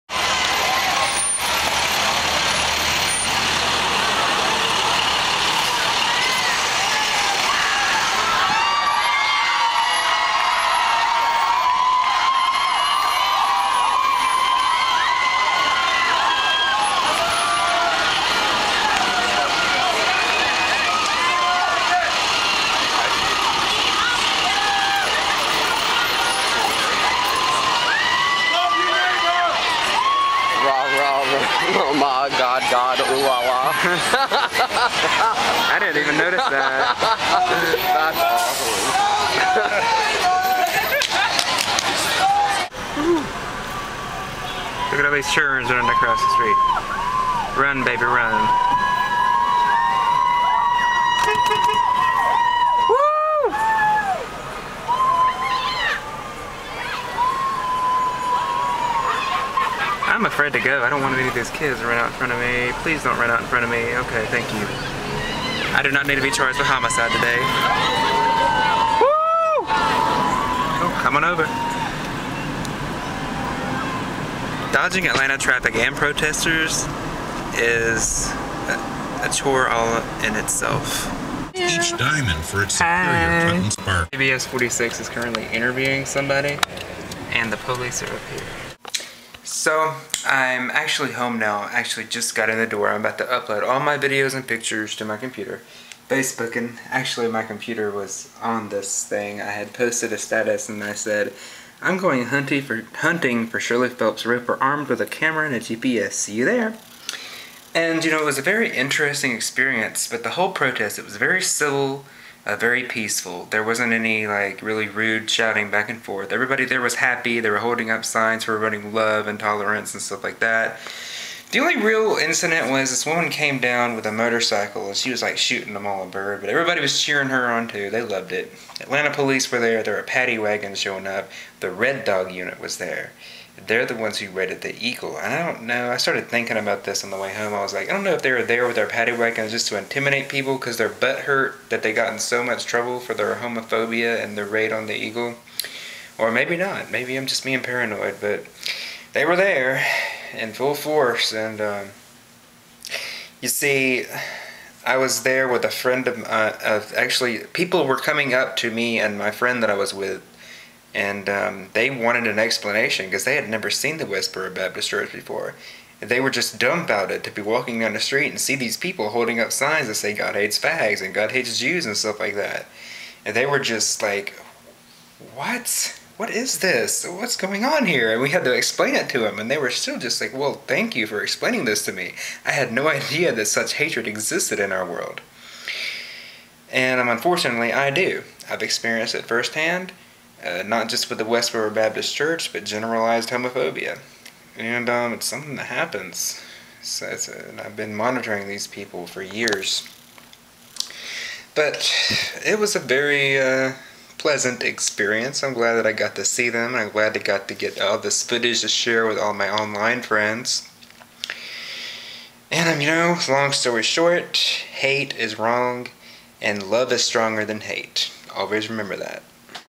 Westboro Baptist Protest in Midtown Atlanta. Part 2
They protested outside of Grady High School on May 6th, 2010. The counter protest was one of peace, love and happiness. We showed them how tolerant the community really is and I'm proud to have witnessed this event.

Love is stronger than H8!

Sorry for having to split this up into 2 parts. I was 1 minute over the 10 min limit but please watch, rate and comment.

-CL =)